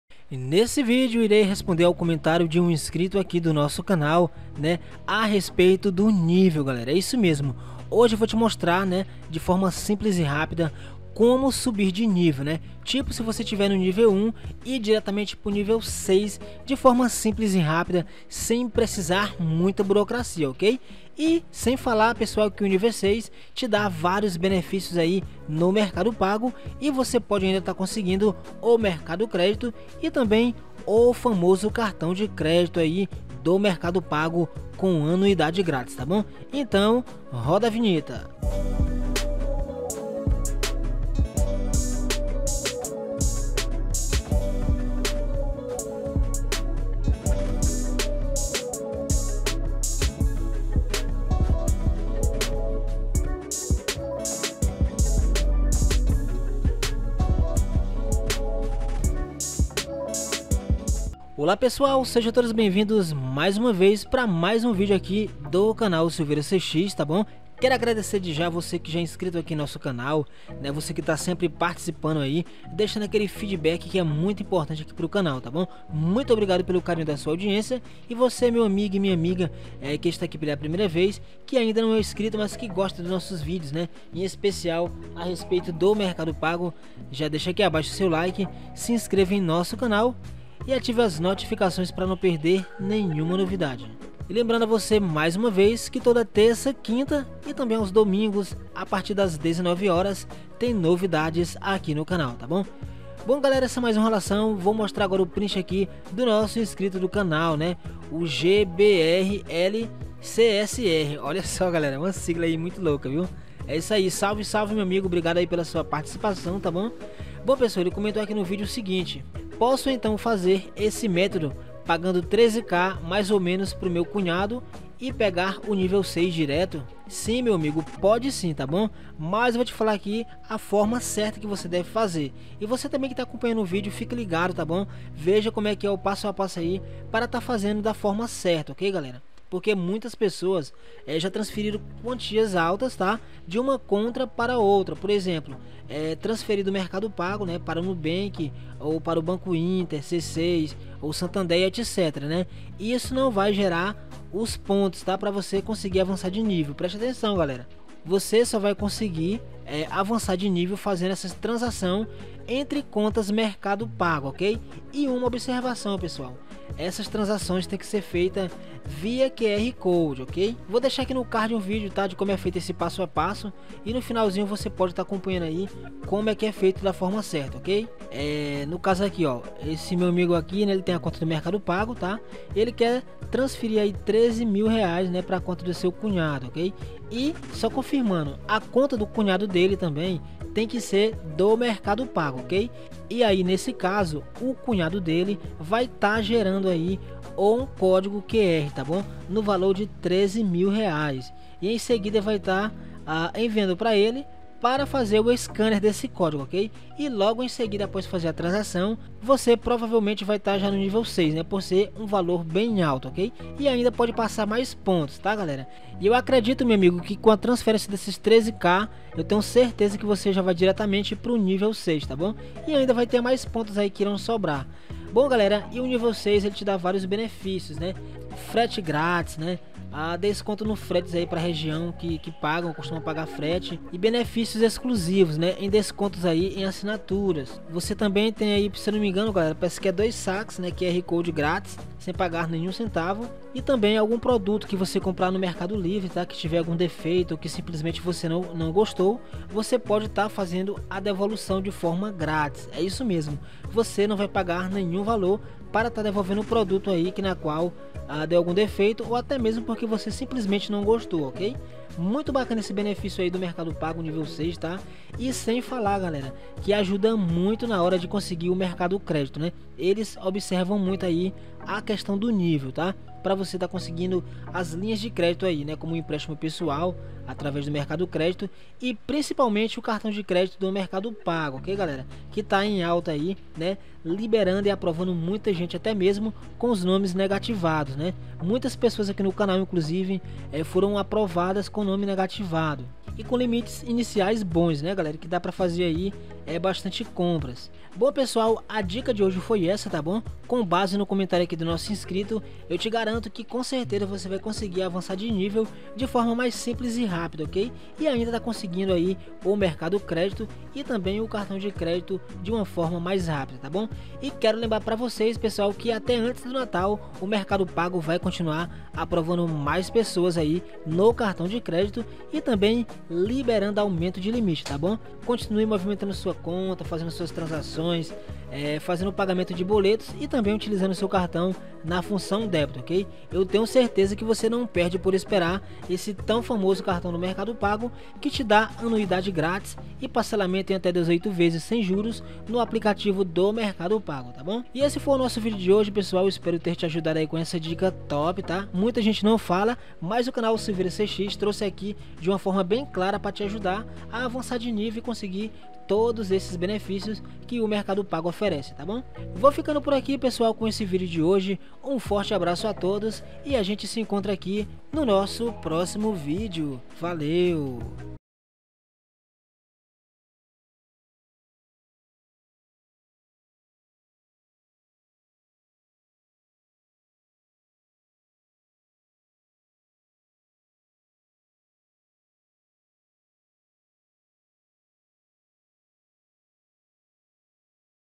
URGENTE!! MERCADO PAGO, FAÇA ISSO E CONSIGA SEU CARTÃO DE CRÉDITO COM MAIS FACILIDADE, CONFIRA...
HOJE TRAGO UMA DICA PARA SER APROVADO RÁPIDO NO CARTÃO MERCADO PAGO, E CONSEGUIR VÁRIOS BENEFICIOS TAMBÉM. ESSE VIDEO É COM BASE NO COMENTÁRIO DE UM INSCRITO AQUI DO CANAL.